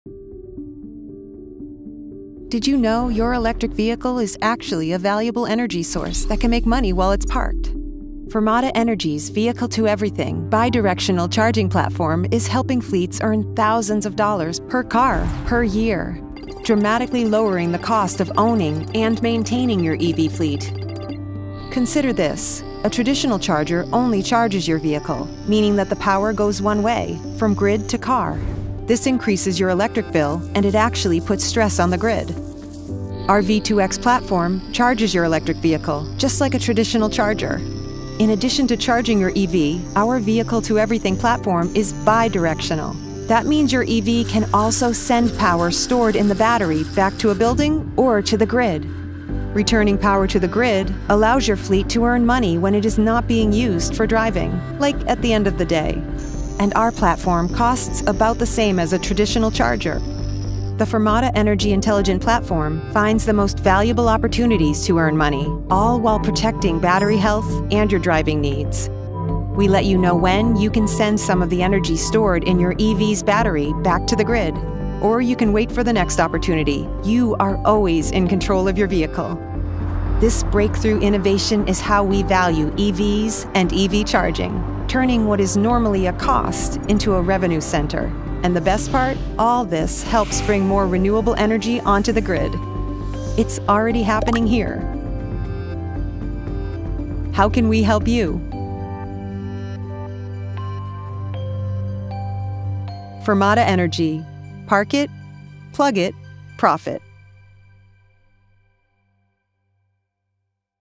V2X bidirectional charging platform from Fermata Energy
With the Fermata Energy V2X bidirectional charging platform, your company can earn revenue while your fleet EVs are parked. Learn more about the positive ROI of V2X, V2G, and V2B. Park it. Plug it. Profit.